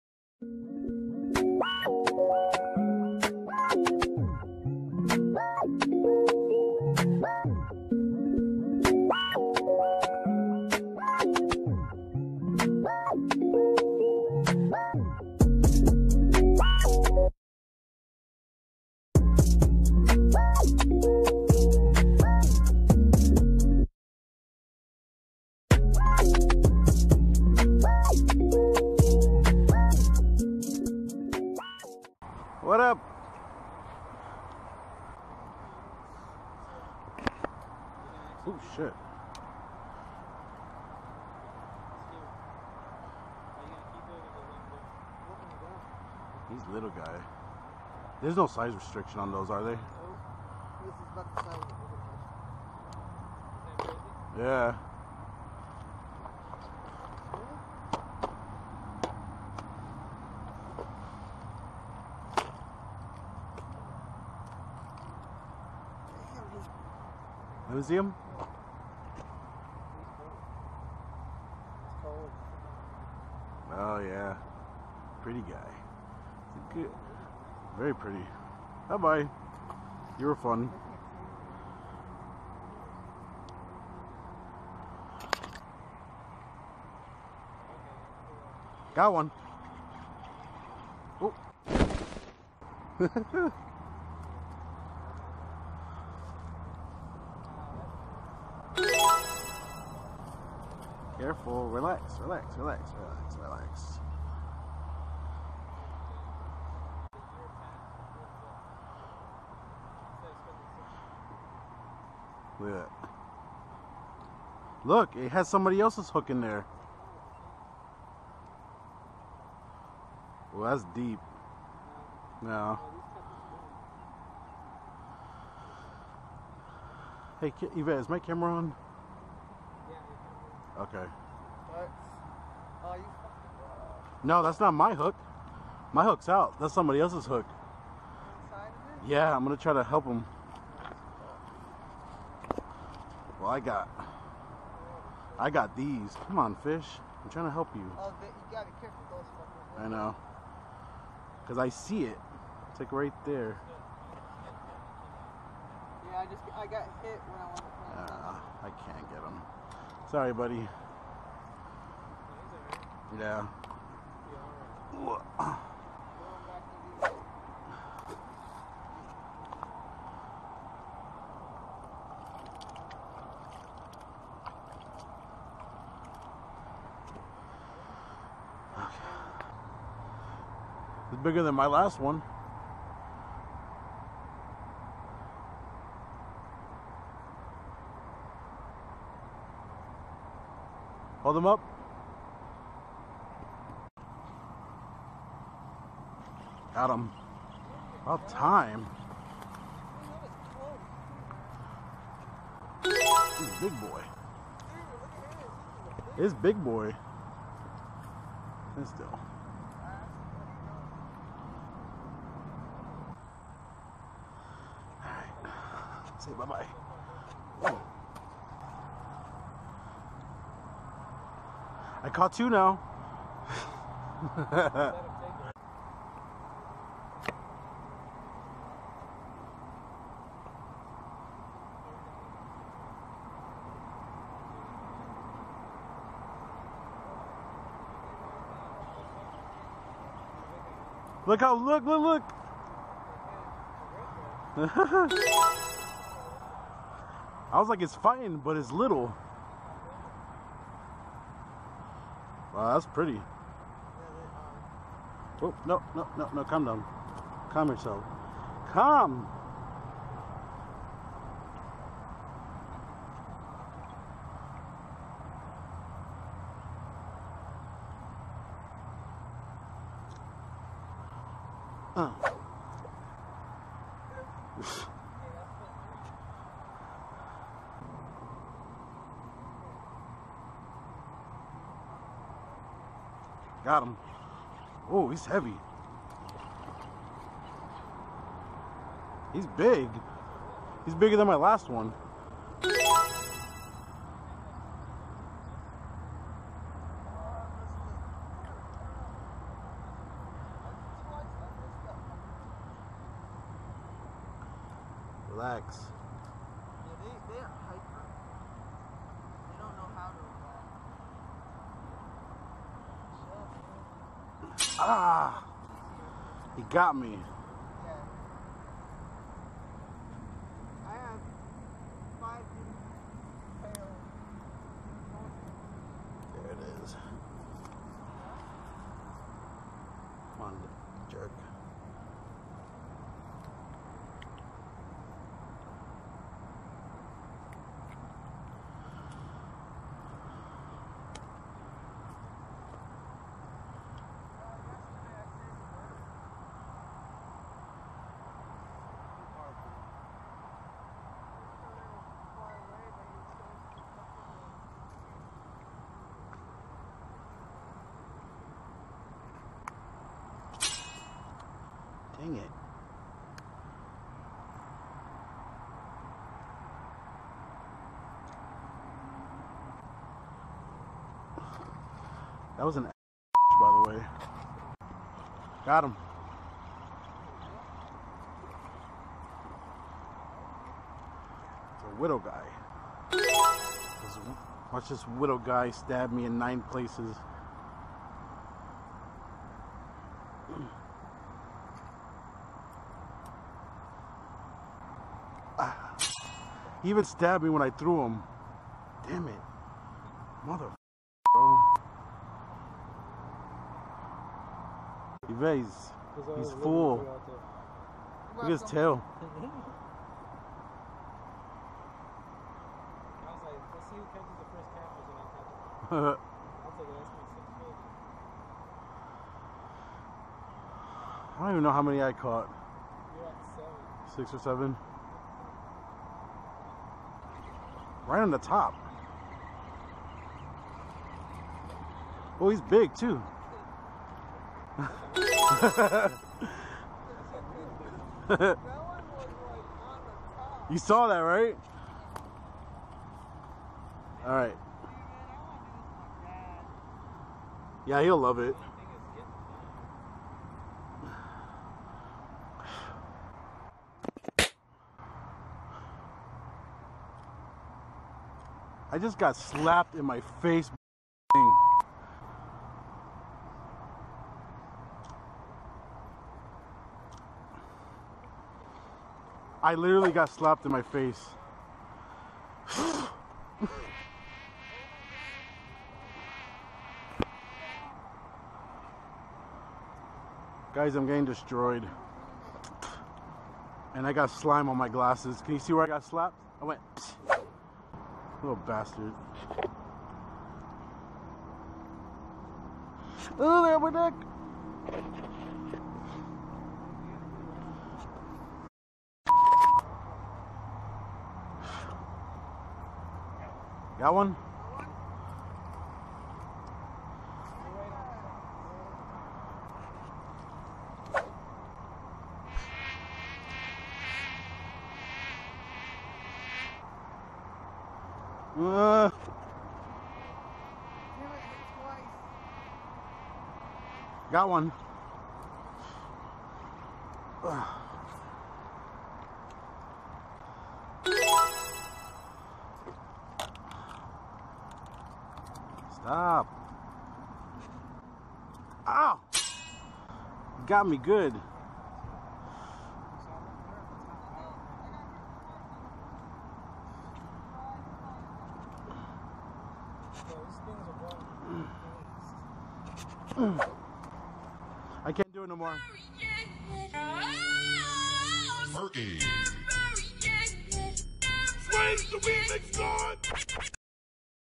Bypass Canal striper hybrid fishing
Did a little evening fishing with my brother and my sister-in-law evefishalots, had an awesome time pulling in these hybrid stripers one after another between the three of us we caught 30 striper that night towards the end I had to give up because the mosquitoes were so bad they're making me insane LOL.